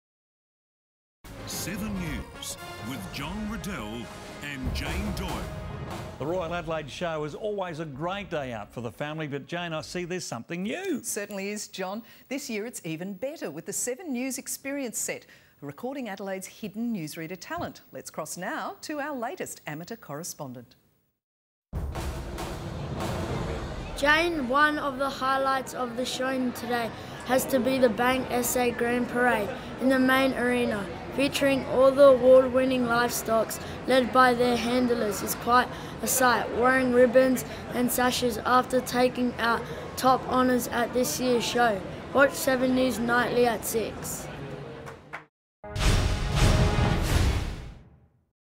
Nicolaou shackal - 7 News Experience
Exclusive report from the Royal Adelaide Show by reporter: Nicolaou shackal